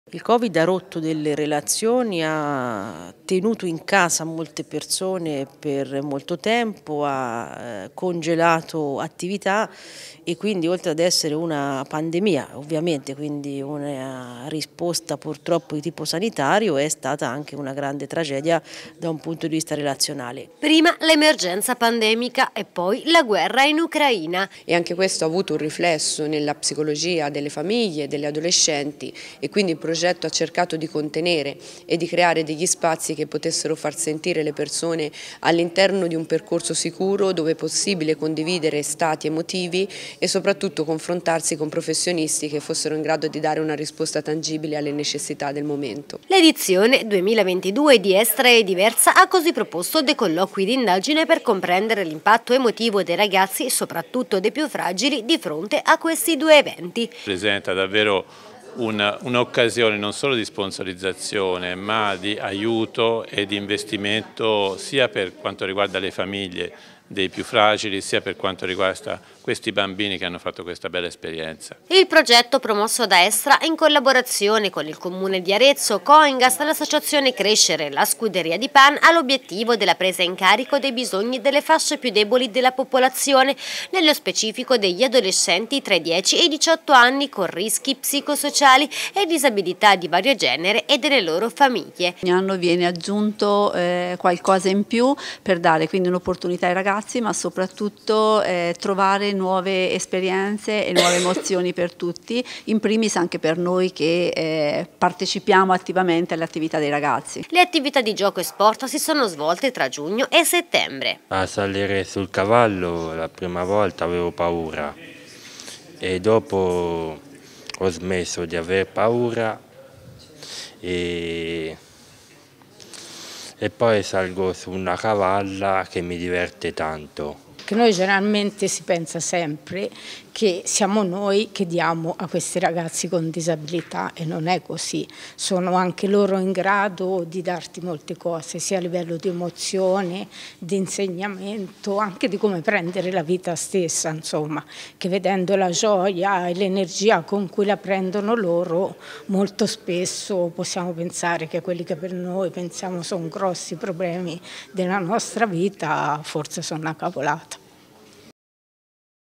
Quale impatto di Covid e guerra sui ragazzi: i risultati del progetto “Estra è diversa”
Come hanno vissuto i ragazzi la fine della fase acuta dell’emergenza Covid? E quali reazioni emotive ha provocato la guerra in Ucraina? A queste domande hanno risposto i colloqui di indagine con i giovani che hanno partecipato all’edizione 2022 di “Estra è diversa”, un progetto ideato e coordinato da ERA, promosso da Estra in collaborazione con Comune di Arezzo, Coingas, associazione Crescere e Scuderia Pan. L'obiettivo era la presa in carico dei bisogni delle fasce più deboli della popolazione, nello specifico gli adolescenti tra i 10 e i 18 anni con rischi psicosociali e disabilità di vario genere e le loro famiglie. Le attività di gioco e sport si sono svolte tra giugno e settembre.